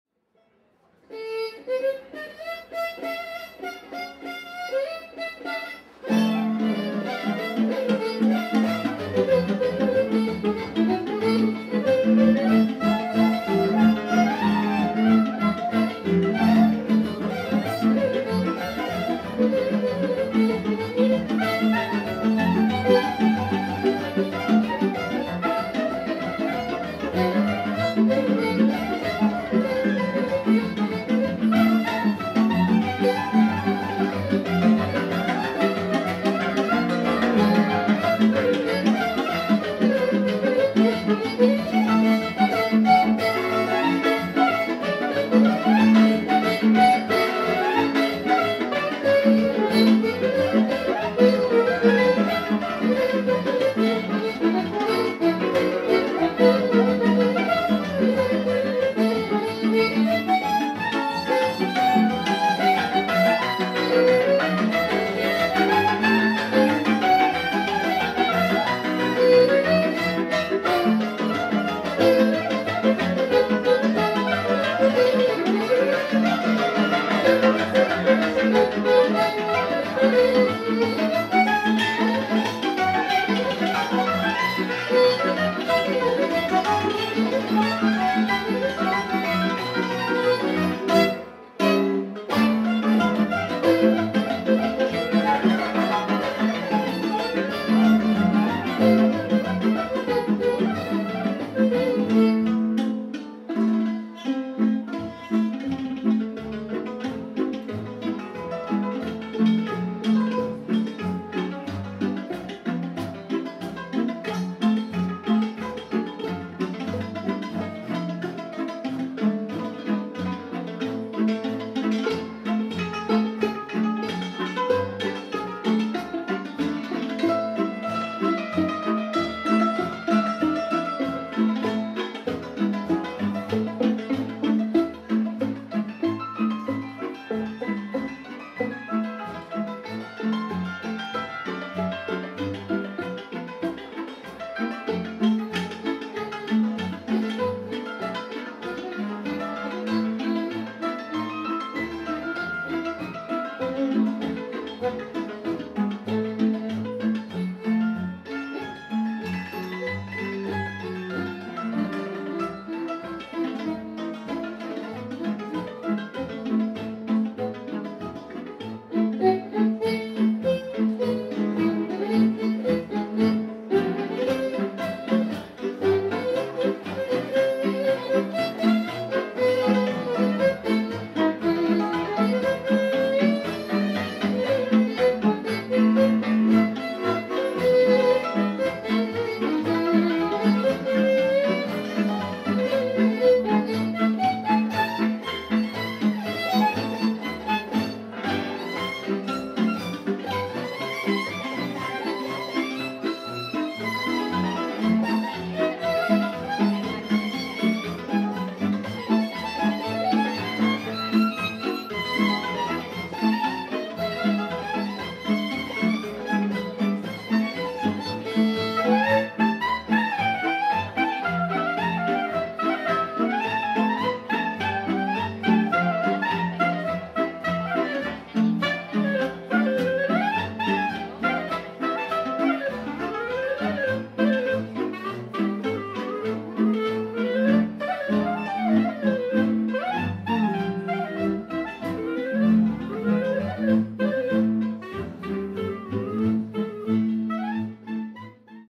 Salamon Beáta és Balogh Kálmán
Méta zenekar Salamon Beáta vezetésével, cimbalom Balogh Kálmán. Bőgő Líber Róbert, fúvós Cserta Balázs, brácsa Fekete Márton. Nyár a Lánchídon fesztivál, 2010.